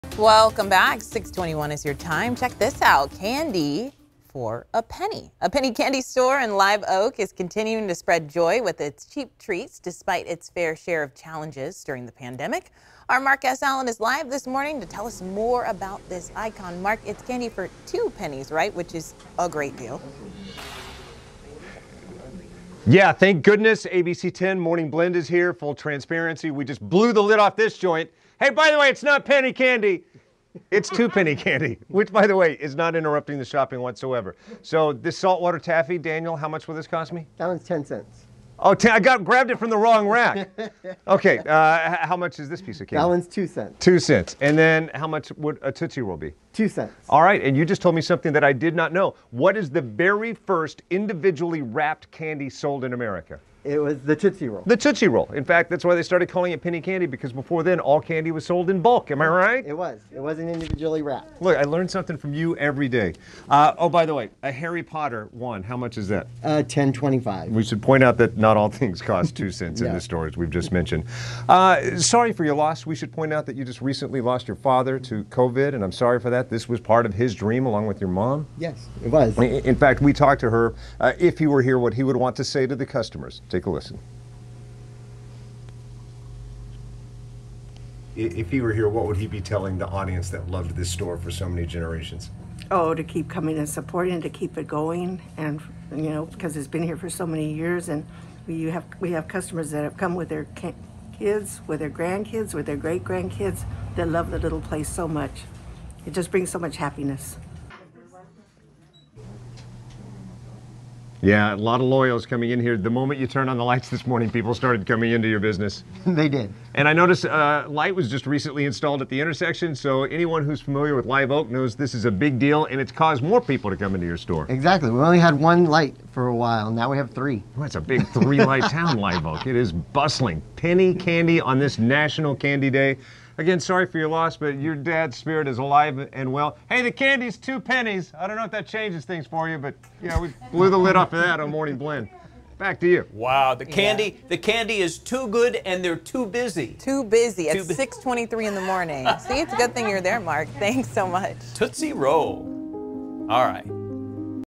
Two-penny candy from Live Oak candy store kicks off National Candy Day
A ton of individually wrapped candies are going for as low as two pennies at this Live Oak candy store in the lead up to Christmas.